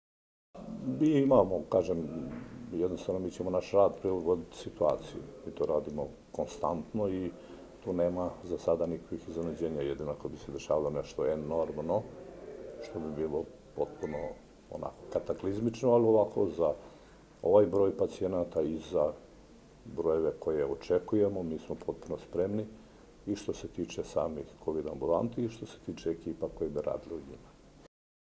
Inđijski Dom zdravlja u pripravnosti da obezbedi još jednu Kovid ambulantu
Dom zdravlja Inđija unazad nekoliko dana u pripravnosti je ukoliko se poveća broj lica sa simptomima virusa Kovid 19 da osposobi još jednu ambulantu. Uz ambulantu u sklopu zdravstvene ustanove Dom zdravlja obezbediće još jednu lekarsku ekipu. Za sada radi samo jedna Kovid ambulanta gde se u toku dana obavi oko 50 pregleda.